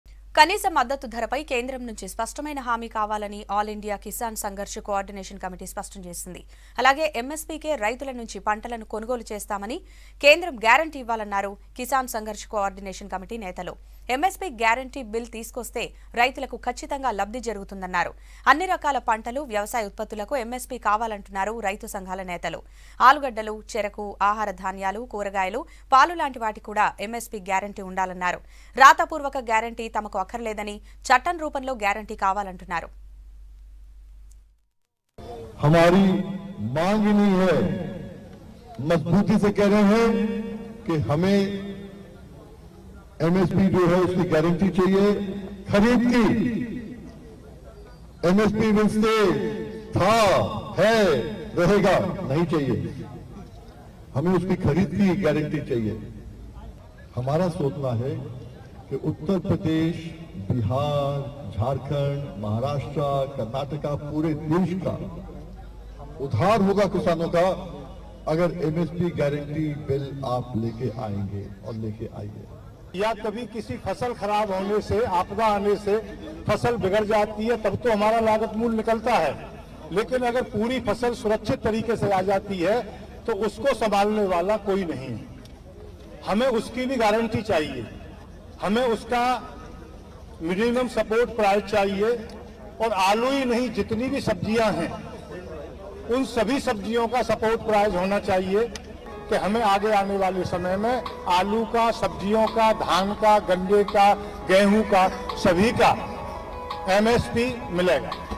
All India Kisan Sangharsh Coordination Committee Demands MSP For Crops | V6 News
All India Kisan Sangharsh Coordination Committee Demands Central Govt To Give MSP For Crops.